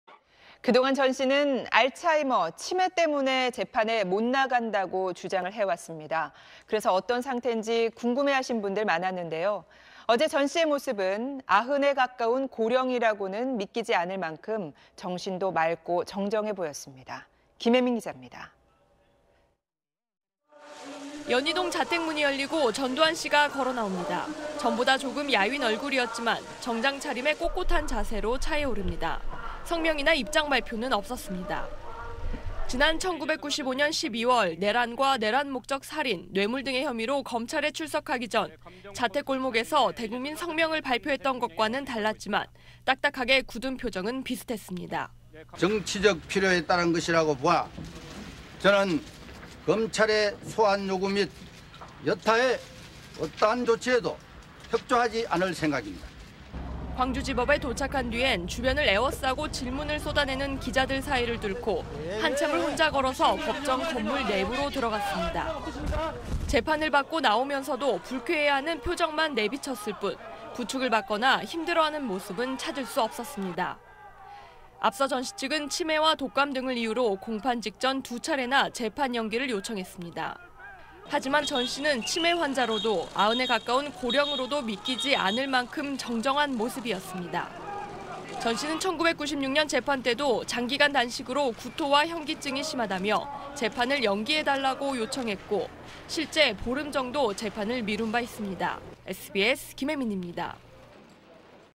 치매라며 출석 피하더니…전두환, 꼿꼿한 자세로 차 탑승 / SBS
그동안 전 씨는 알츠하이머, 치매 때문에 재판에 못 나간다고 주장해왔습니다. 그래서 상태가 어떤지 궁금해하신 분들 많았는데요, 어제(11일) 전 씨의 모습은 아흔에 가까운 고령이라는 게 믿기지 않을 만큼 정신도 맑고 정정해 보였습니다.